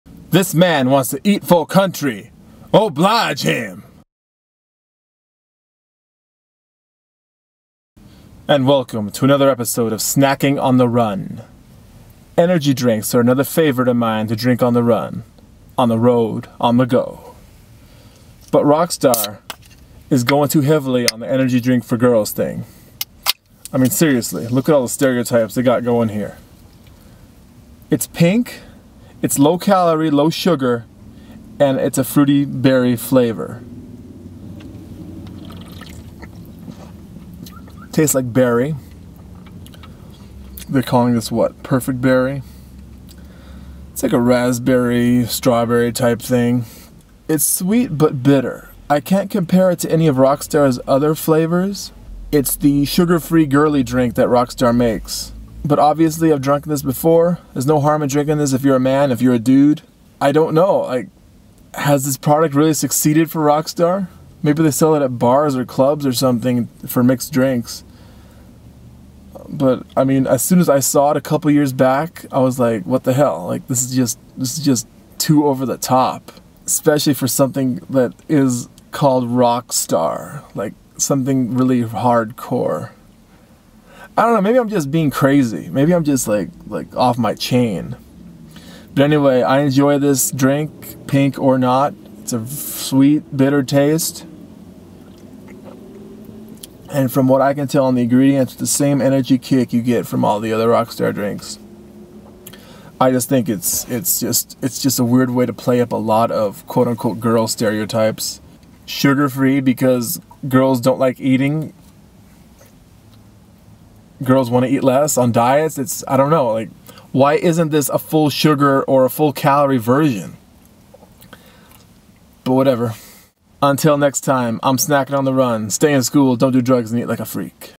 Rockstar Pink - Energy Drink for Girls? (Snacking On The Run Ep 3)
Did Rockstar Energy go too far when they designed their sugar free berry drink? Is it trying too hard to be a girly drink, or is my imagination crazy? Pink Rockstar indeed.

All LIKES, SHARES, COMMENTS & FAVORITES greatly appreciated. Eat like a freak!

Filmed in Yucca Valley. No product endorsement or placement here.